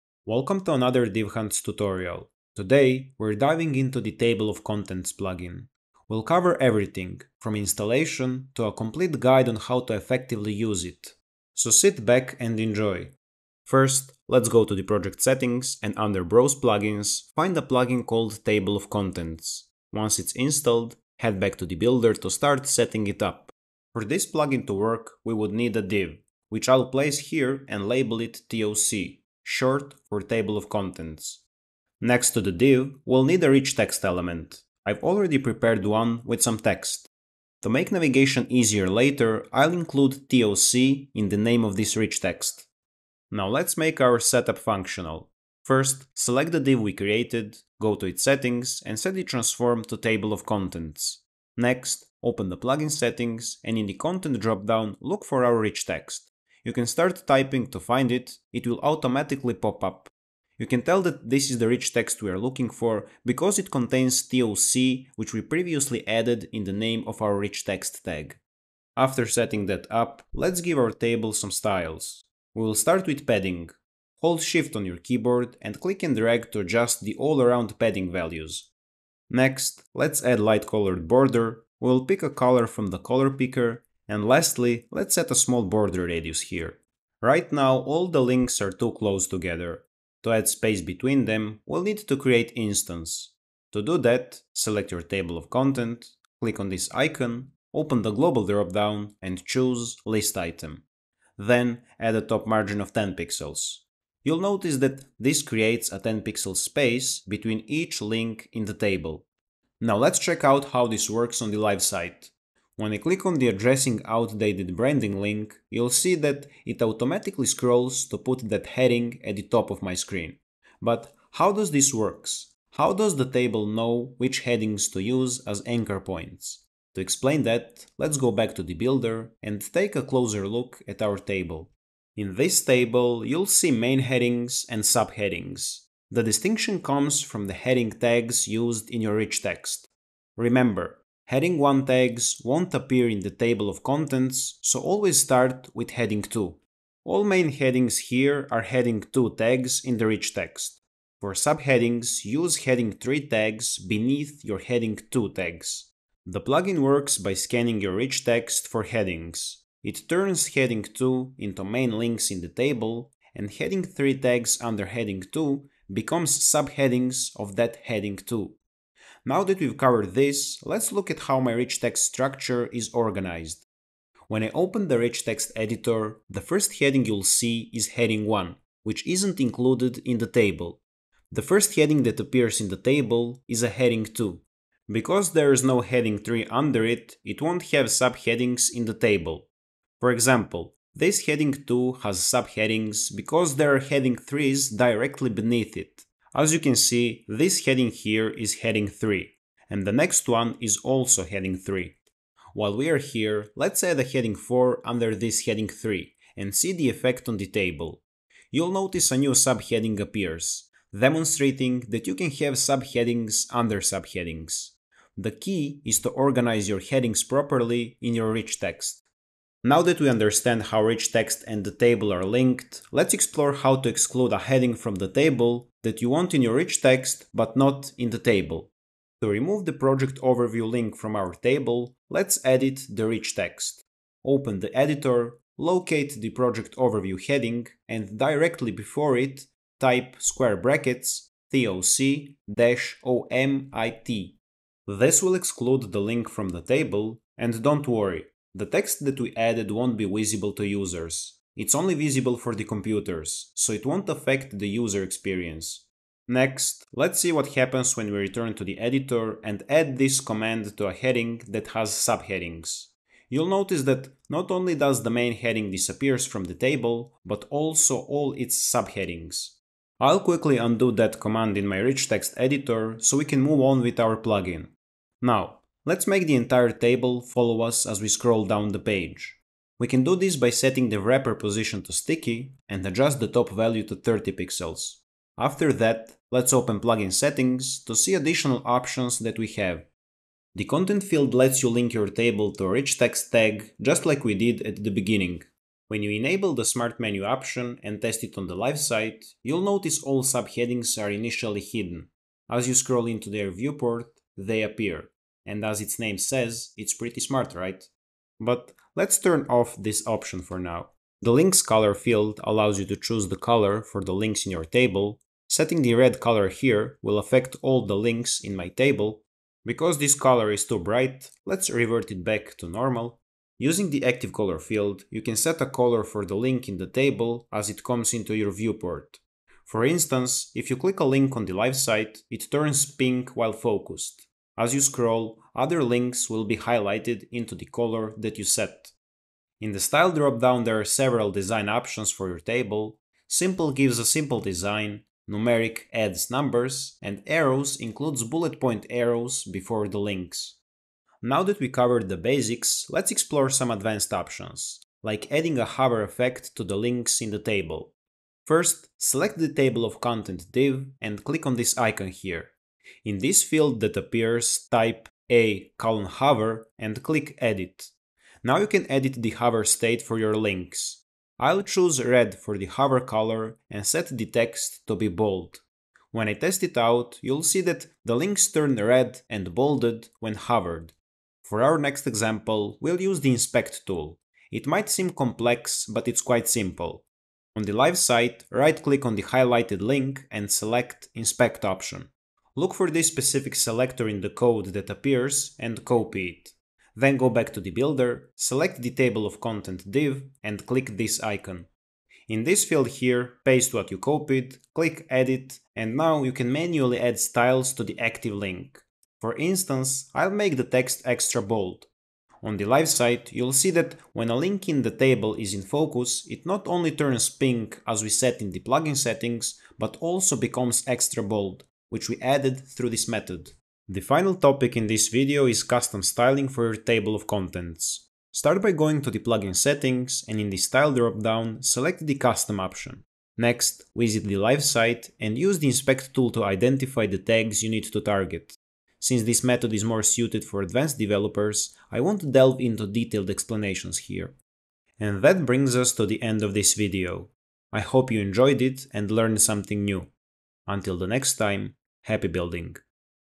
Create Table of Content For Your Blog Post Without Code | Divhunt Plugin
Learn how to create a Table of Content for your blog posts, or any other content pages that require better and easier navigation. Set up everything in just a few clicks and easily, without writing a single line of code.

Faster than the greatest no-code tools, and just as limitless as building from scratch - welcome to Divhunt.